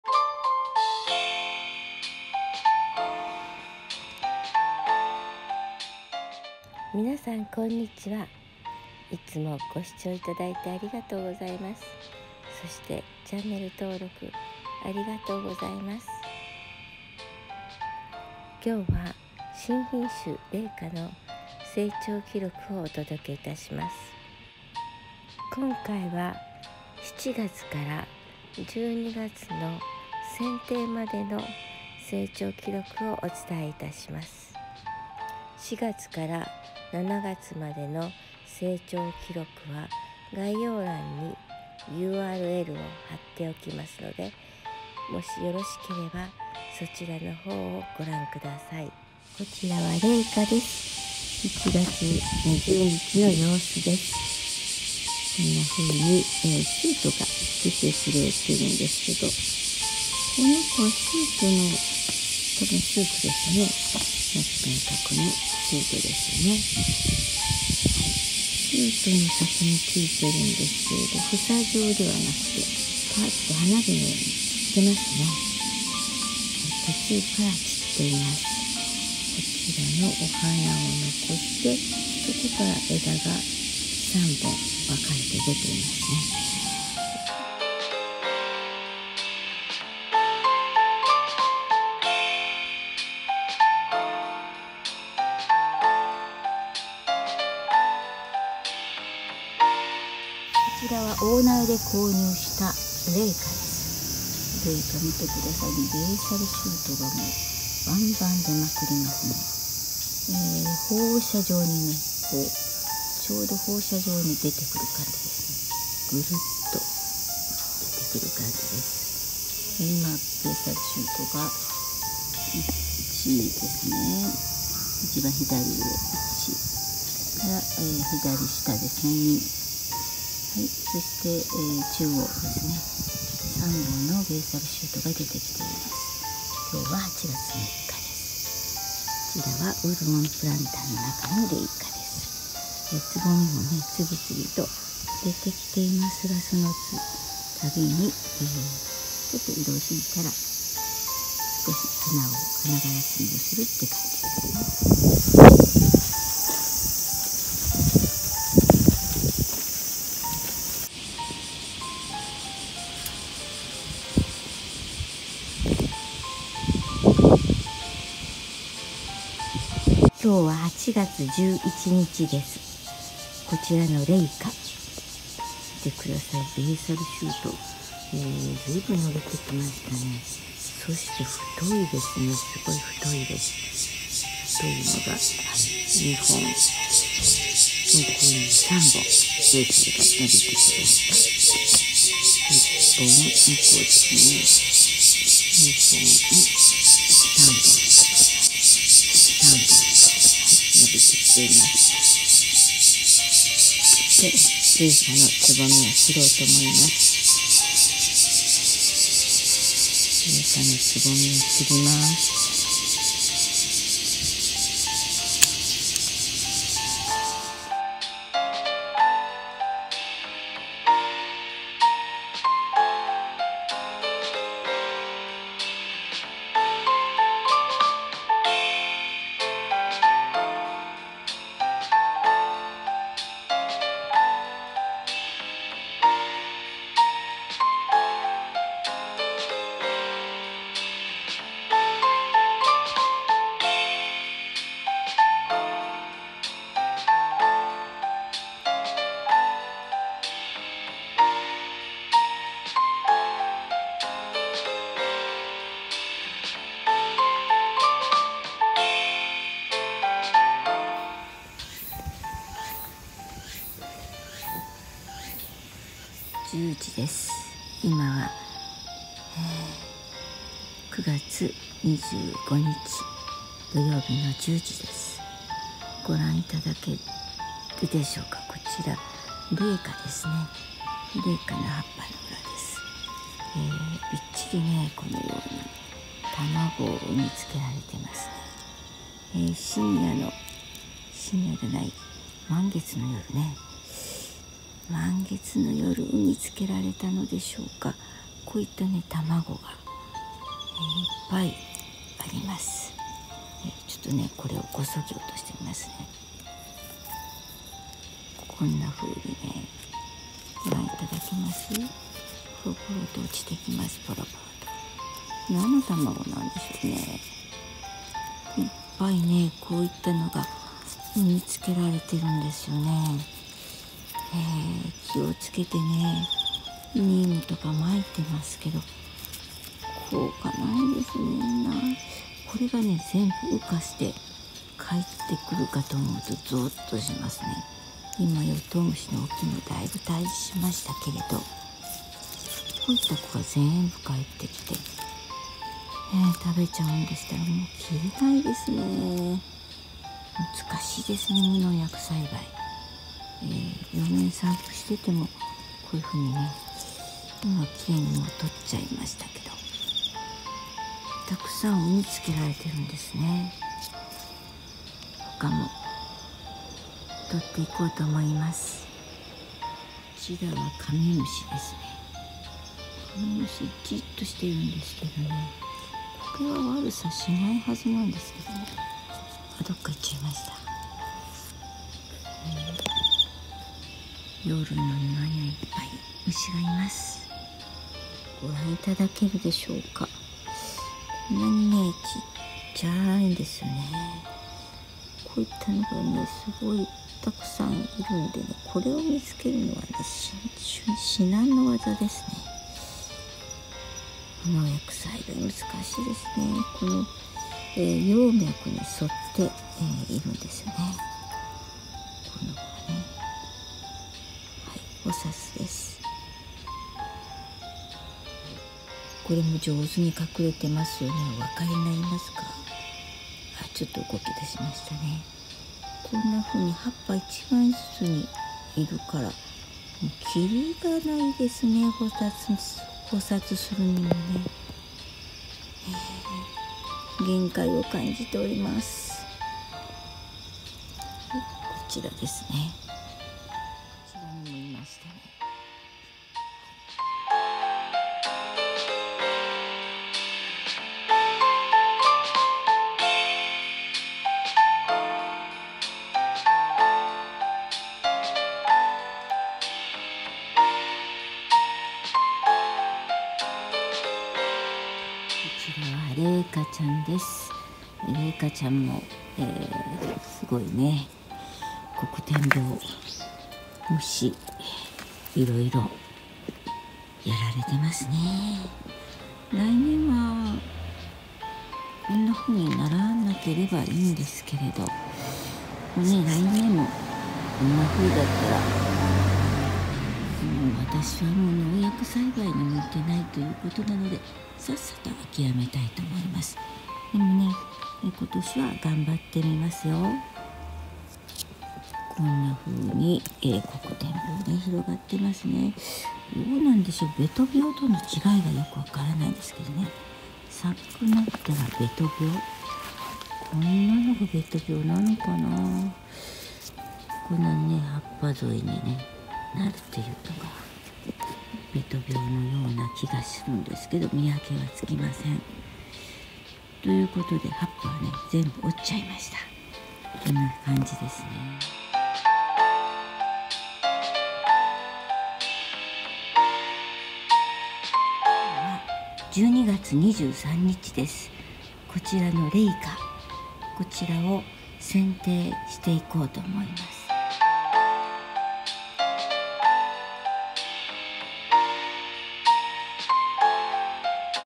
Fragrance rose新品種の香りの薔薇 令香の成長記録です。まだまだデーターが少ないから記録動画を撮ったらどうか？と越後丘陵公園の元ガーデナーさんのアドバイスで制作しました。7月〜12月です。
新品種香りの薔薇　令香の成長記録
4月〜7月